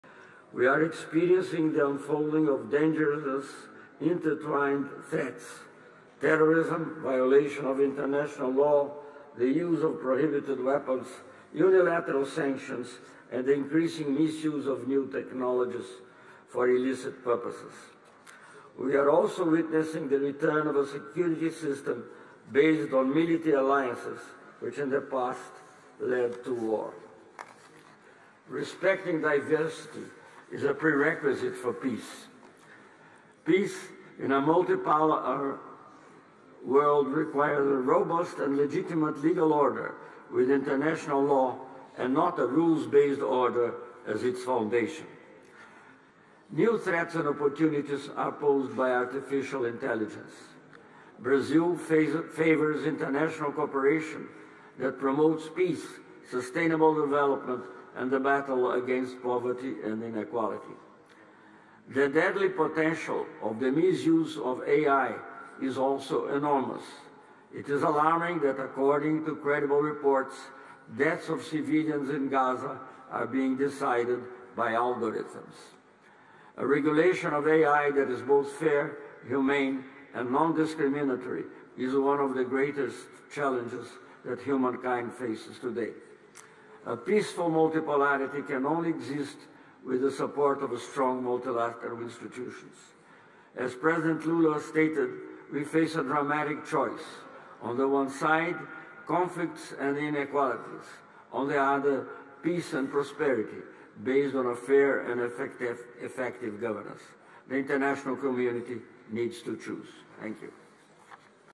Sem citar OTAN, Amorim diz na Rússia que alianças militares já levaram à gu3rra
Sem citar Otan, Celso Amorim diz, na Rússia, que sistema de segurança baseado em alianças militares, no passado, levou à gu3rra.

Assessor para assuntos internacionais de Lula participou de reunião do Conselho de Segurança russo, na quarta-feira (24).